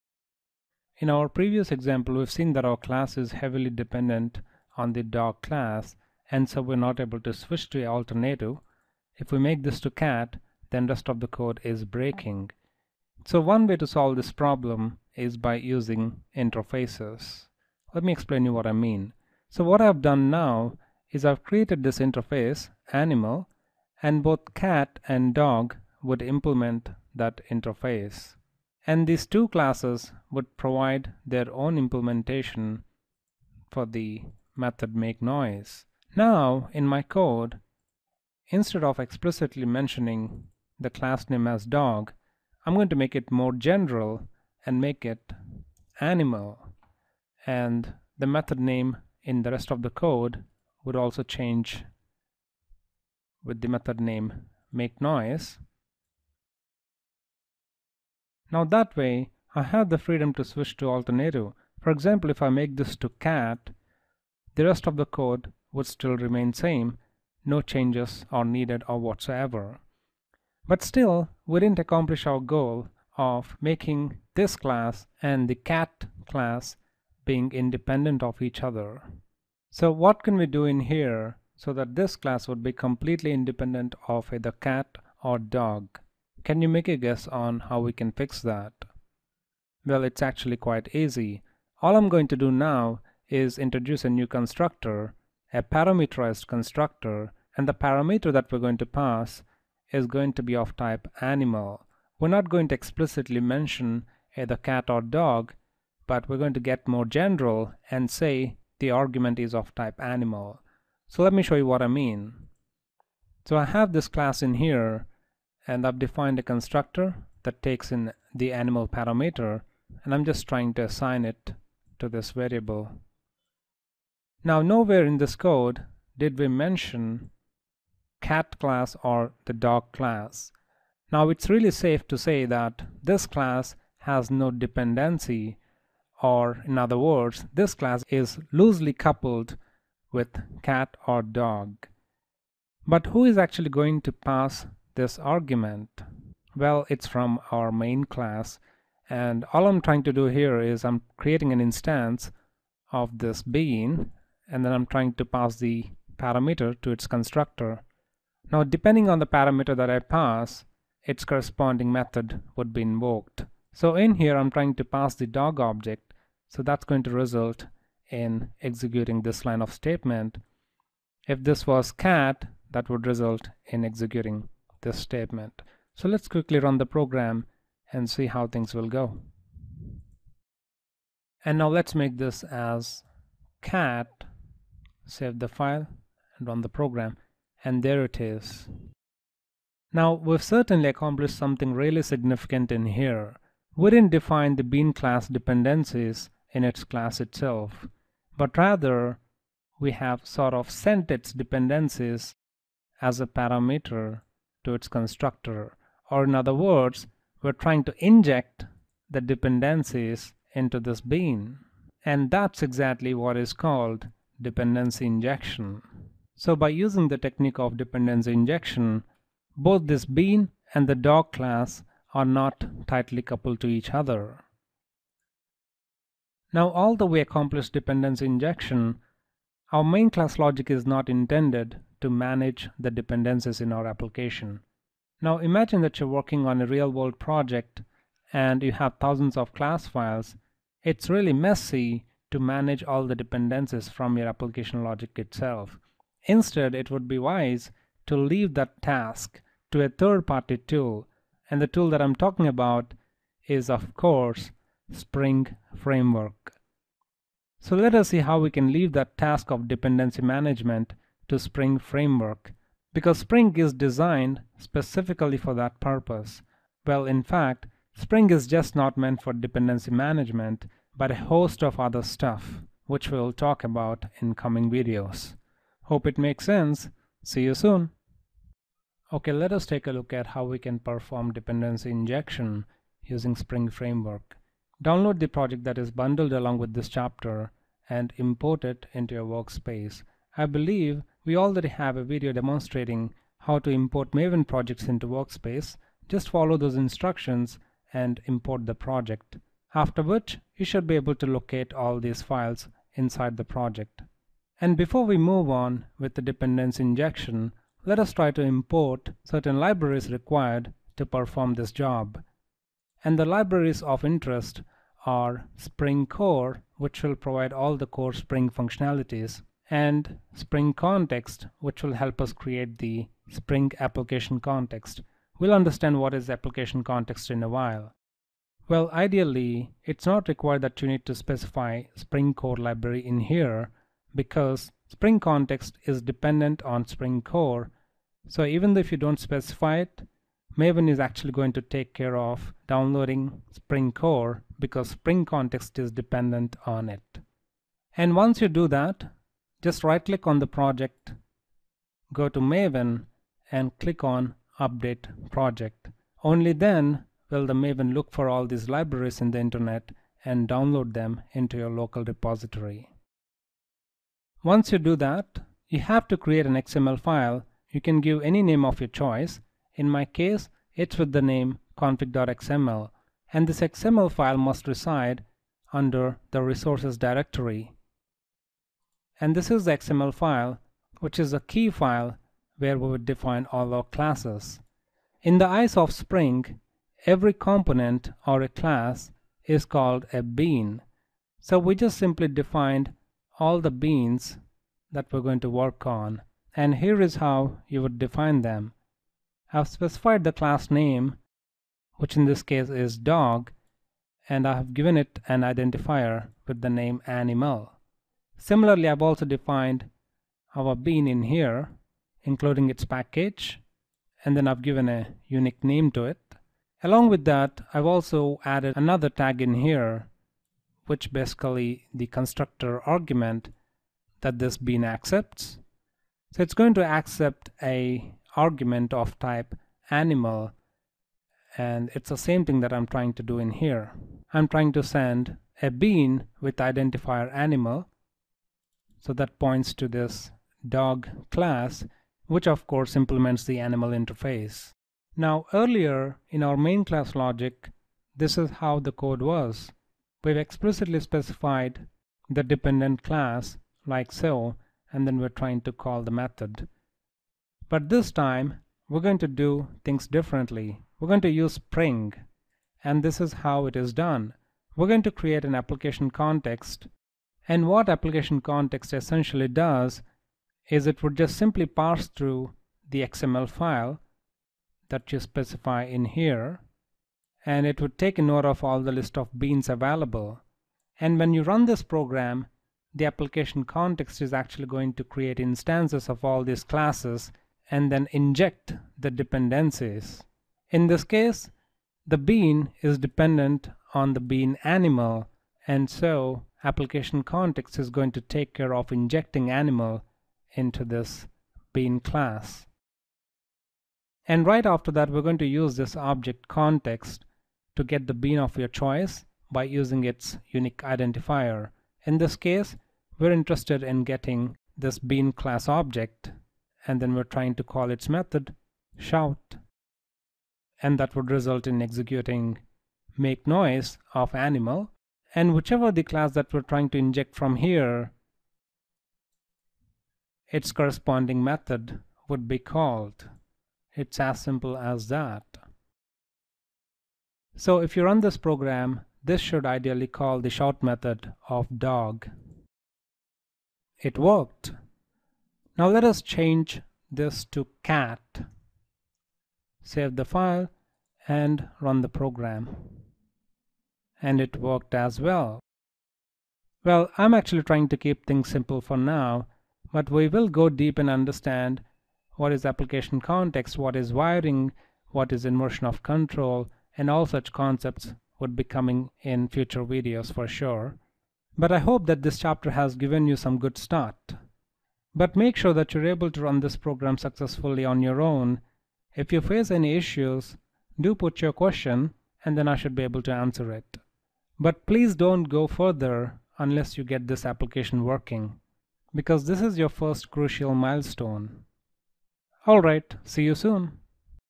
Dependency Injection in Java and Spring with Example - Explained in easy way form karpado.com
Dependency injection refers to the process of injecting the dependencies by an external container with inversion of control mechanism. To understand more, watch the video.. Links & Resources!